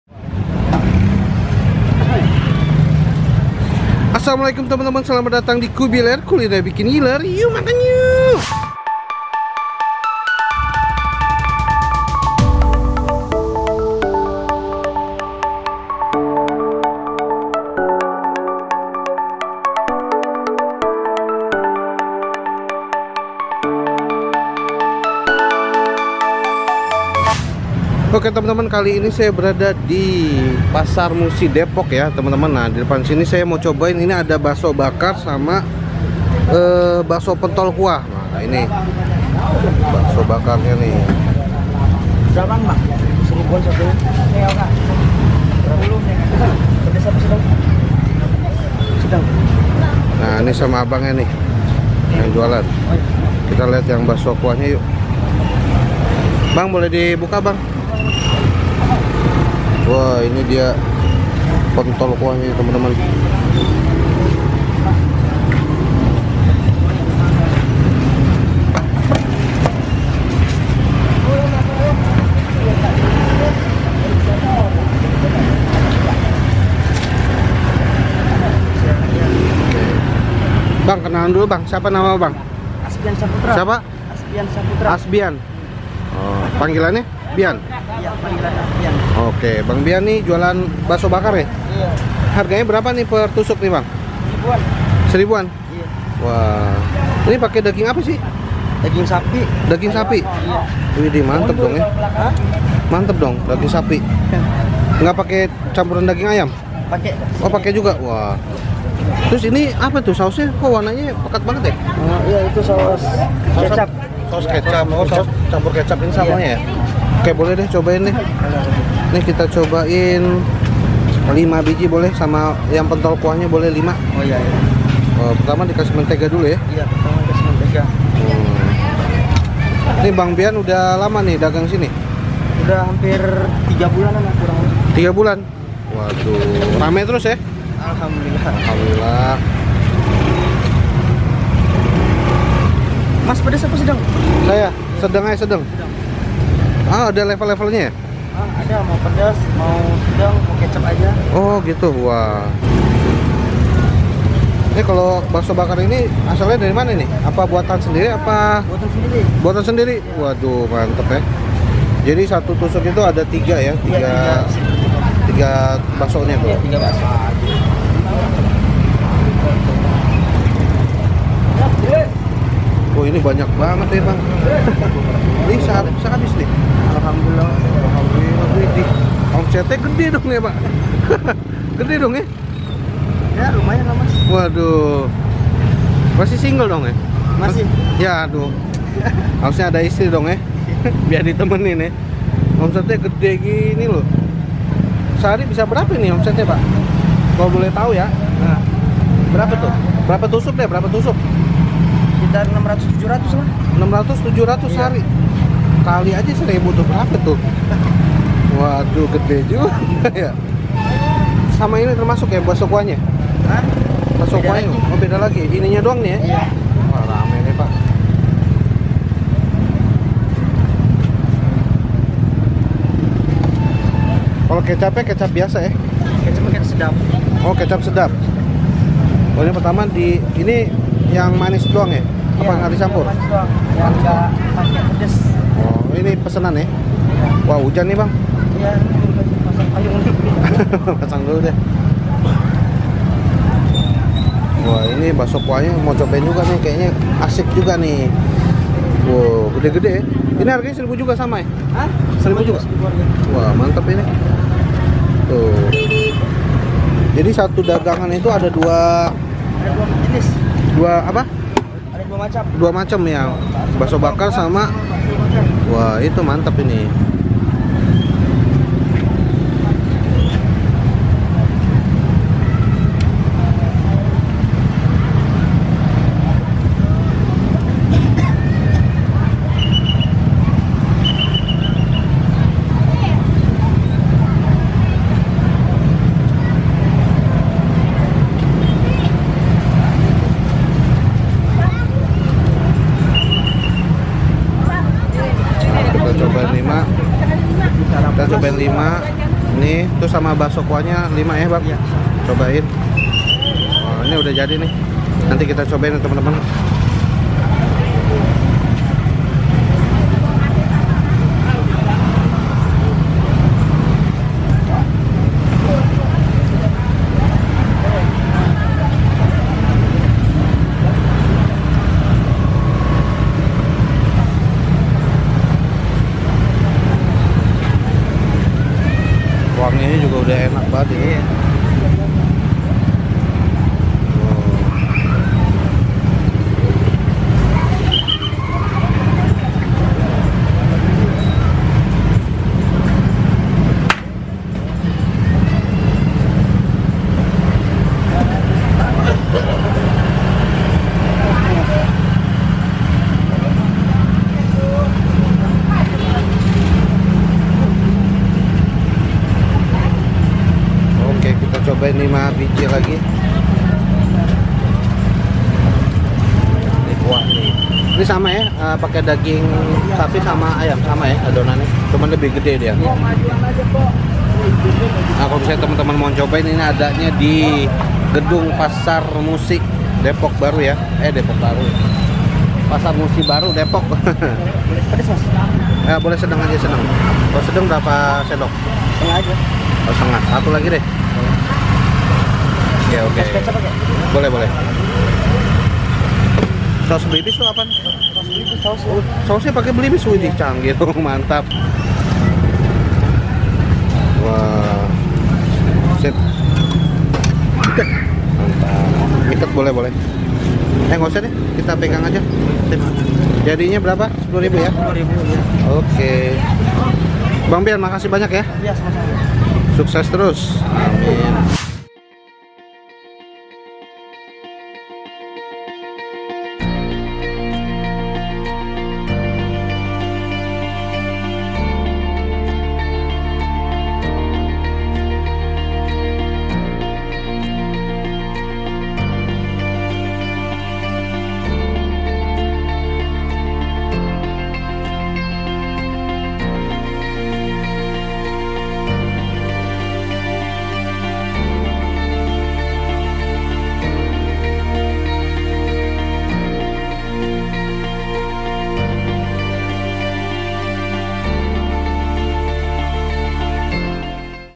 CUMA JUALAN BAKSO BAKAR & PENTOL KUAH !! OMSET BISA MENCAPAI PULUHAN JUTA BULAN
Hallo temen-temen
Selamat datang di CHANNEL KUBILER, Kuliner Bikin Ngiler
"Yuk Makan Yuk"

BAKSO BAKAR DAN PENTOL KUAH  !!!

HARGA : 1.000
BUKA : PAGI